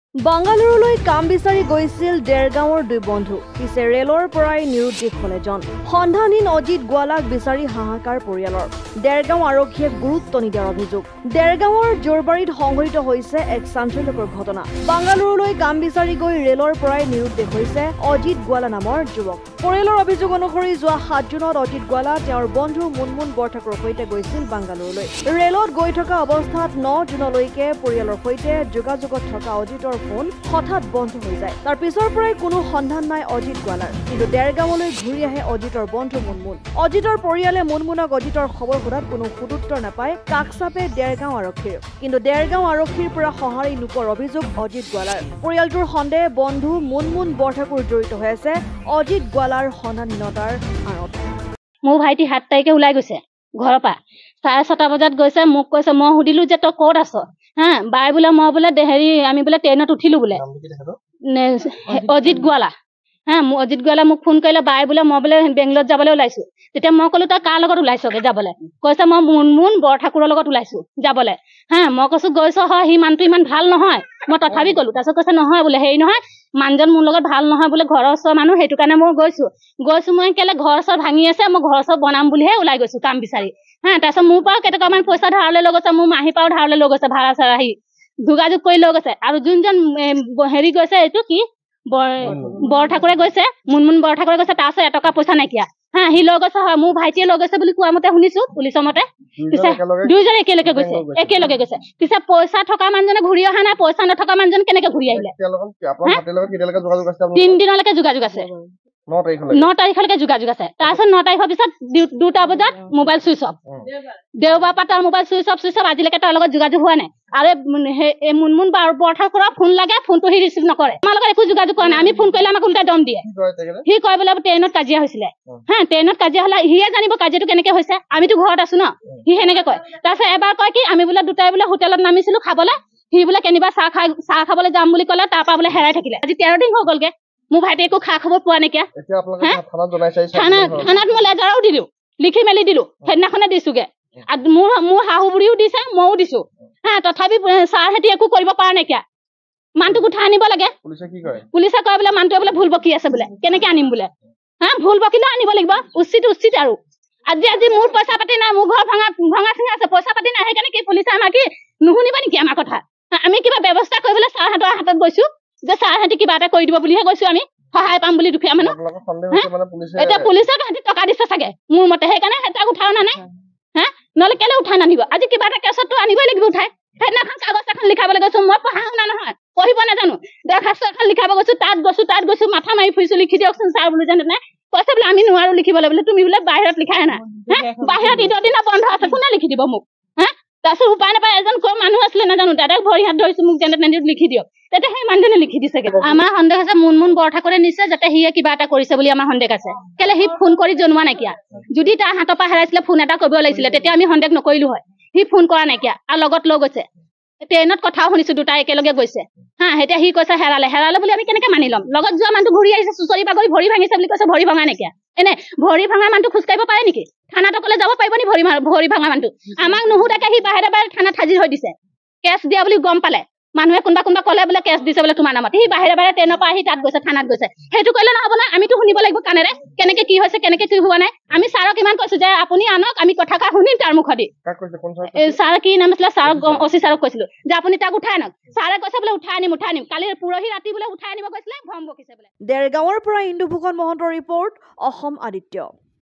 দেৰগাঁৱৰ যোৰবাৰীত সংঘটিত হৈছে এক চাঞ্চল্যকৰ ঘটনা
বাংগালোৰলৈ কাম বিচাৰি গৈছিল দেৰগাঁৱৰ দুই বন্ধু, পিছে ৰে'লৰ পৰাই নিৰুদ্দেশ হ'ল এজন, সন্ধানহীন অজিত গোৱালাক বিচাৰি হাহাকাৰ পৰিয়ালৰ, দেৰগাঁও আৰক্ষীয়ে গুৰুত্ব নিদিয়াৰ অভিযোগ